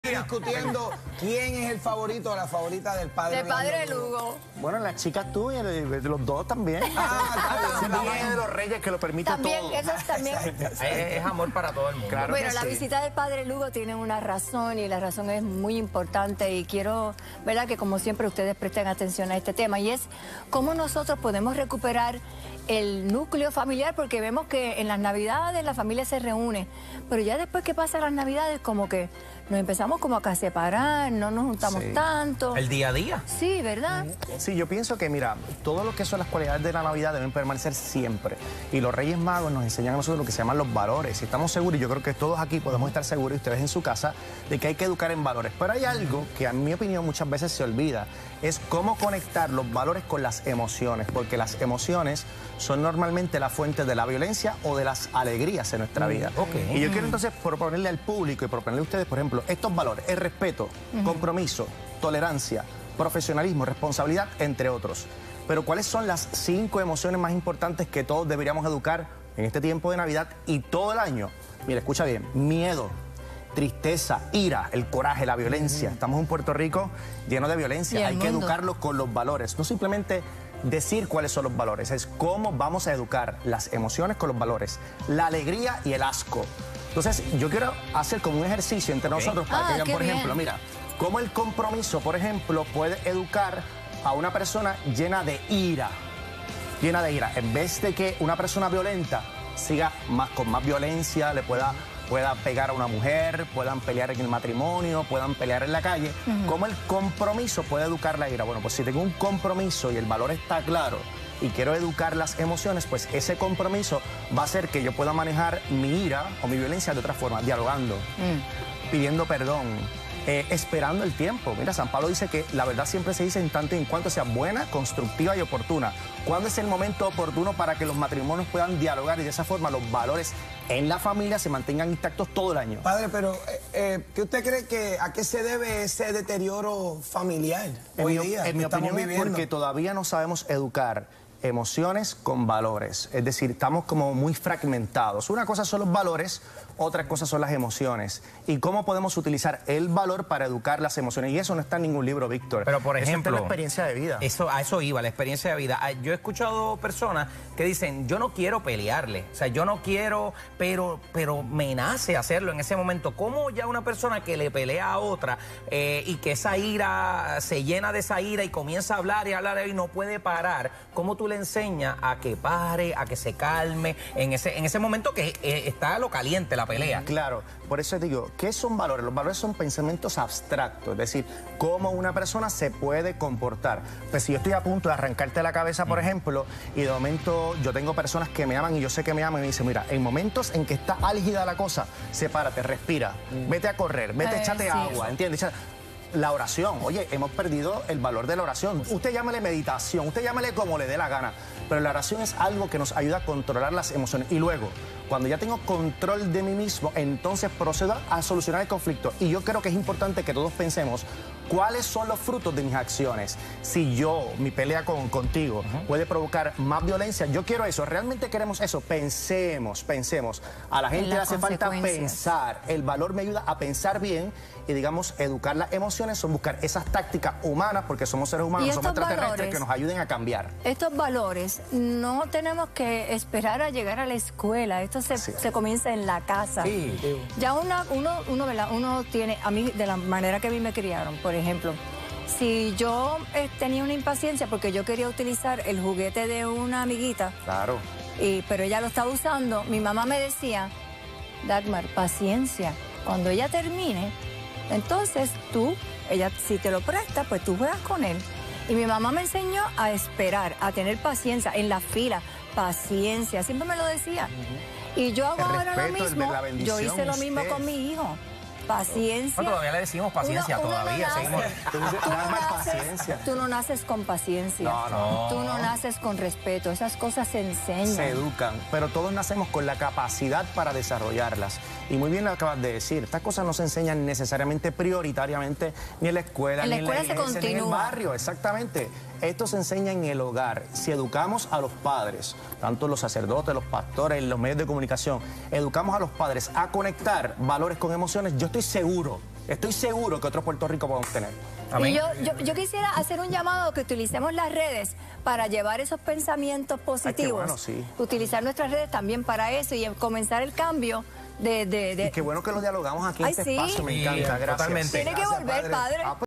¿Cómo recuperar el núcleo familiar?
El padre Orlando Lugo hace énfasis en los valores y cómo conectarlos con las emociones.